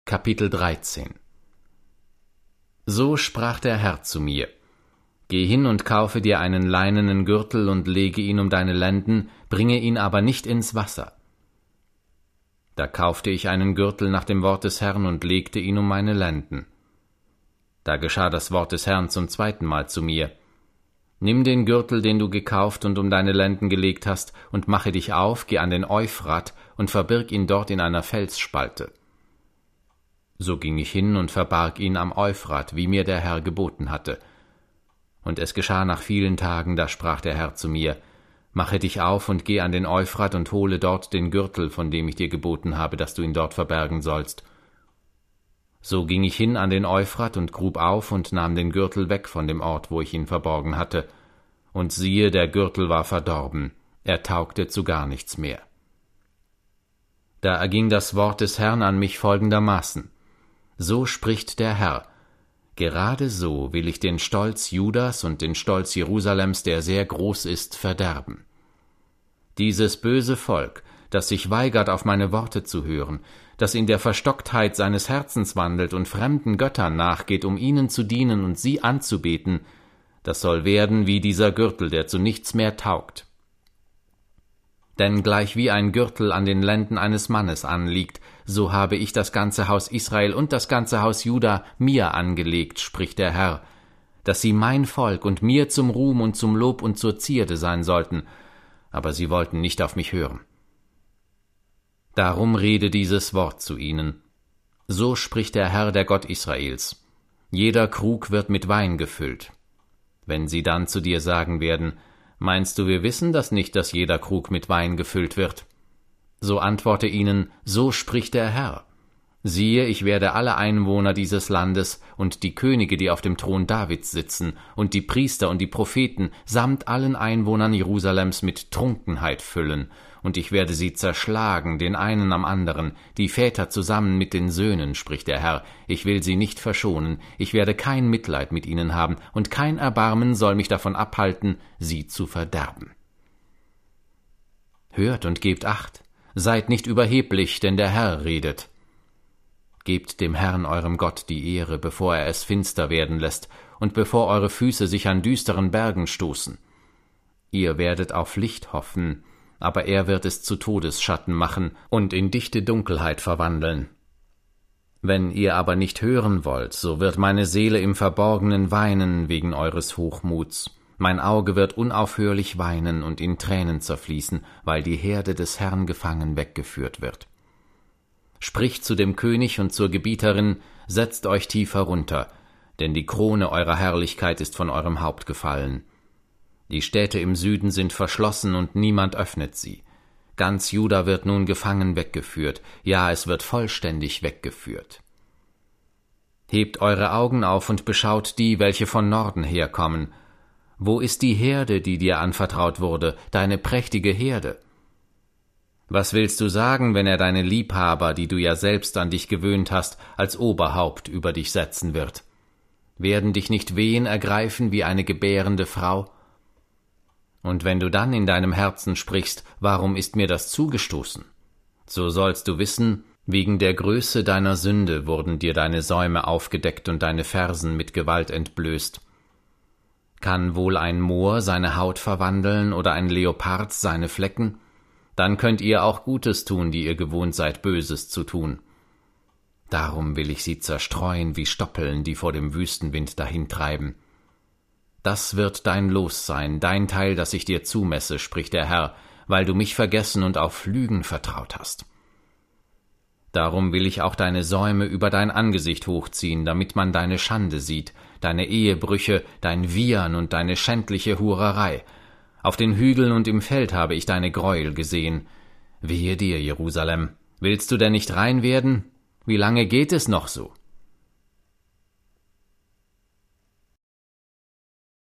Das Buch des Propheten Jeremia - Kapitel 13
Das Zeichen des verdorbenen Gürtels
Die Weinkrüge
Ankündigung der Gefangenschaft für Juda